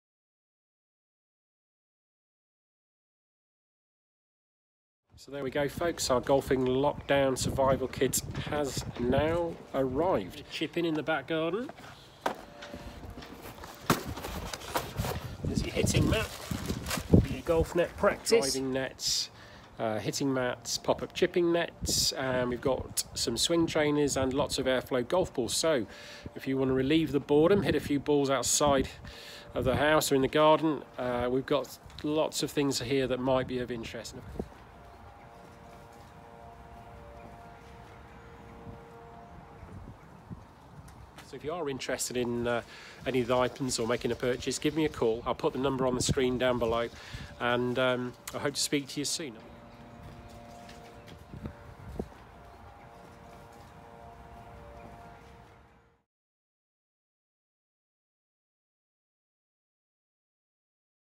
Lockdown Golf Practice Training Aids
Golf retailers are now able to operate a click-and-collect service where goods are pre-ordered and either collected off the premises or delivered.